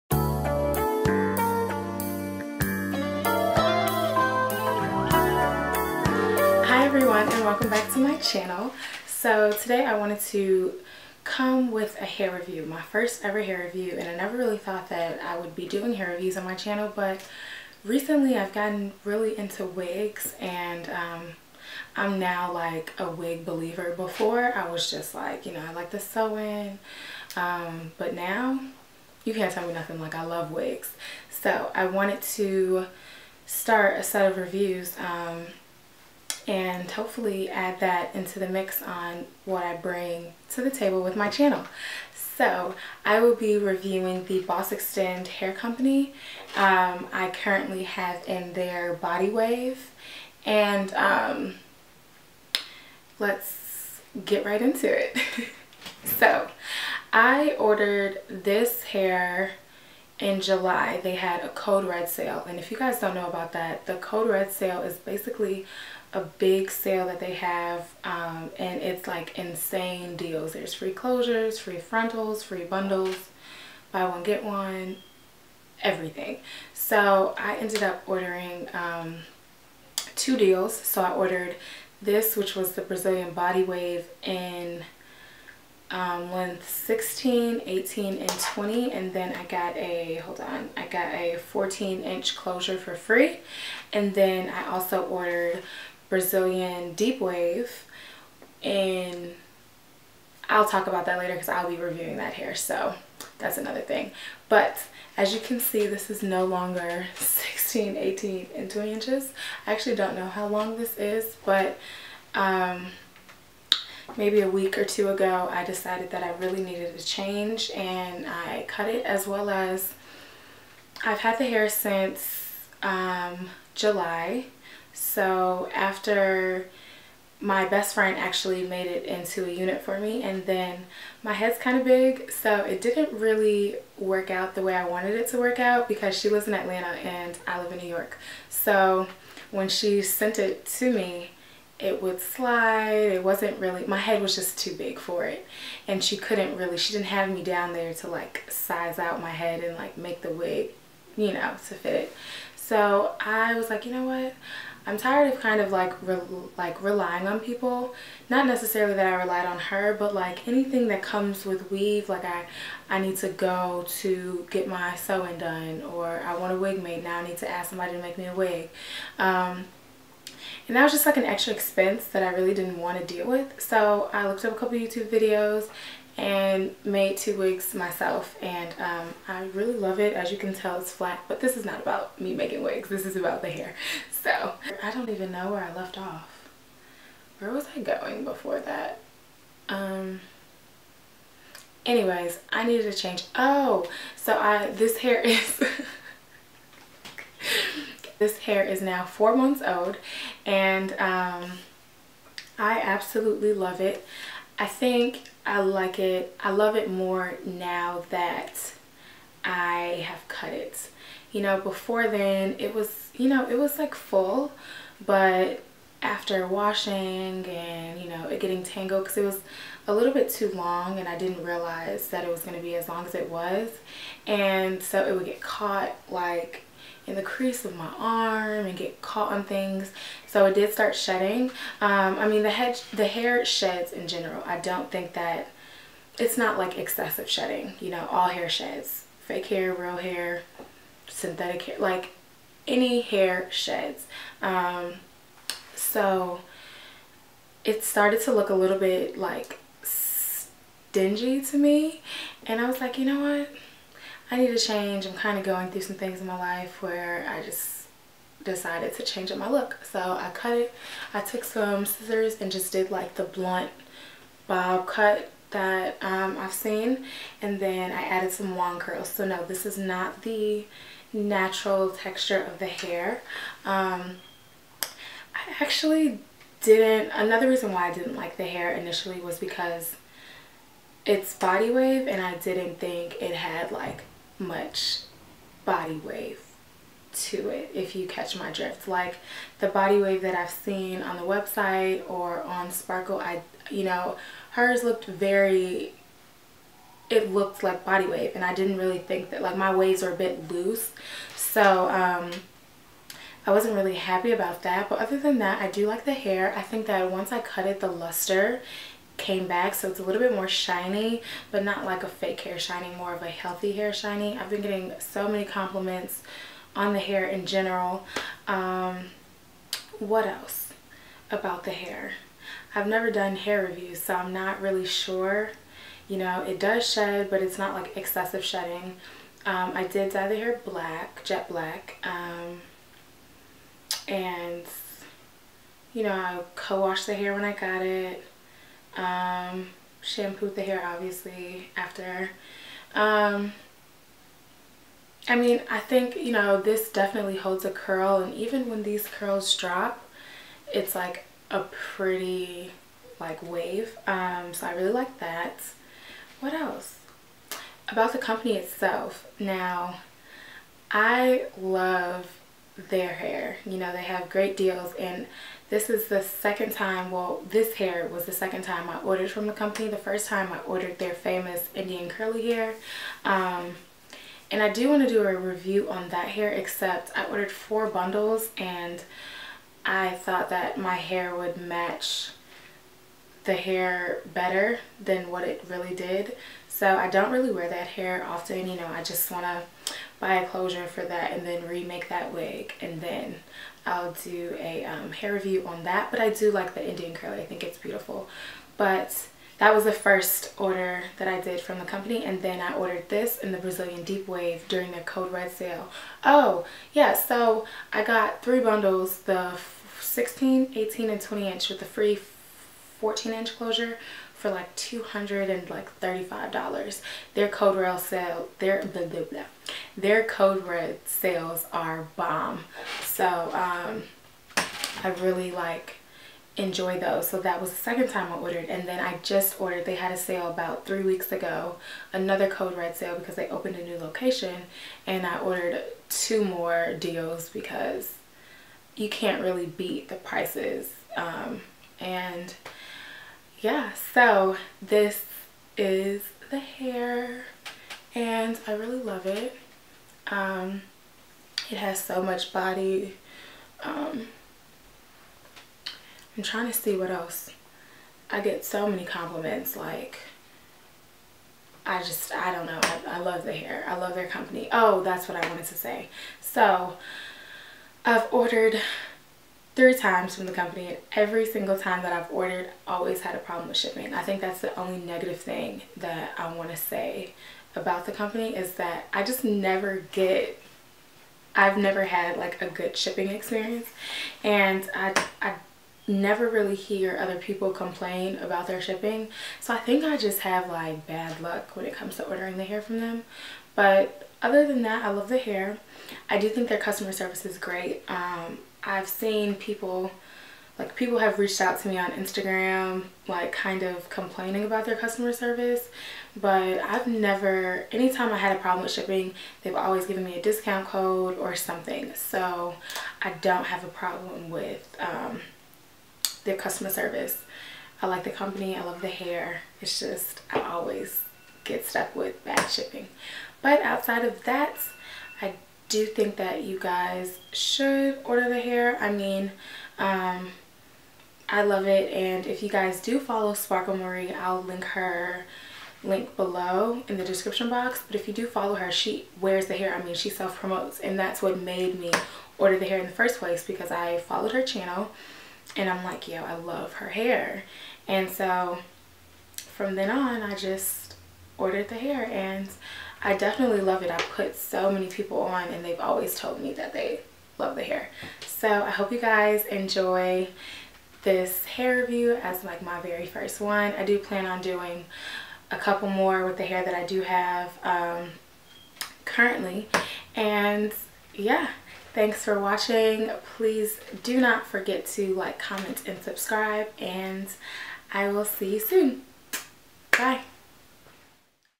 BossxTend Brazilian Body Wave Hair Review
I am back with a new video except this time it is my first ever hair review! I am reviewing the Brazilian Body wave hair from BossxTend!